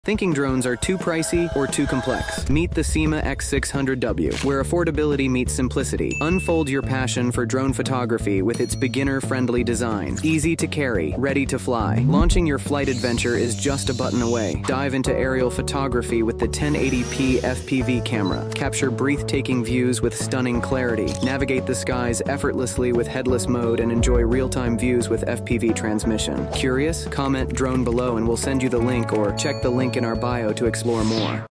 Elevate Your LEISURE Time ⭐ SYMA X600W Drone - Your Ticket to BREATHTAKING Aerial Shots 📸
🎥🚁 Get ready to soar high and capture breathtaking views with the SYMA X600W Foldable 1080P FPV Camera Drone! Perfect for all your leisure and hobby adventures, this drone is a fusion of smart solutions and effortless efficiency. 🌟

In this video, we're taking you to new heights with the SYMA X600W, showcasing its innovative features. Witness how this drone's 1080P camera delivers crystal-clear aerial footage, making every flight an unforgettable experience. The foldable design ensures ultimate portability, so you can take your hobby anywhere with ease.

🛠️ Key features include:
Foldable & Portable Design
1080P FPV Camera for Stunning Clarity
Effortless Control with Stable Hovering
Extended Flight Time for Longer Adventures

📺 Don't forget to watch how we unfold the potential of this amazing drone.

Discover more about our innovative products and how they can transform your leisure time: